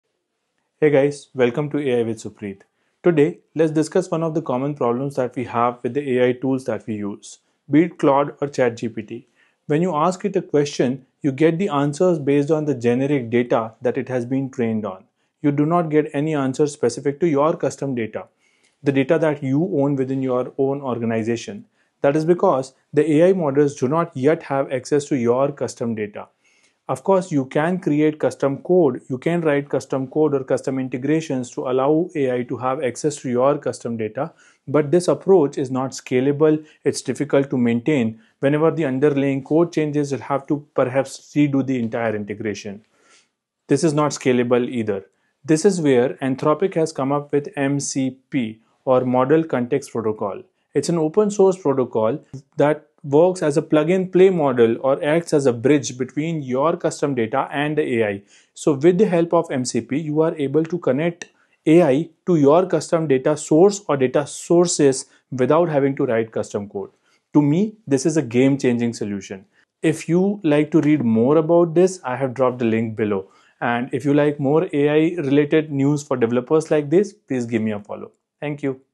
Anthropic MCP - Model Context Protocol
Up until now, you’ve been writing custom code to give AI access to your data. But that might not be needed for much longer.

Anthropic’s newly introduced solution, MCP (Model Context Protocol), is set to change the game for good.

MCP acts as a universal bridge between your data and AI, offering a plug-and-play approach that eliminates the need for custom integrations. Whether it’s your databases, APIs, or documents, MCP standardizes how AI models interact with your specific datasets.

If this innovation gets the adoption it deserves, we’ll soon be able to have AI seamlessly interact with our data—with no custom coding required.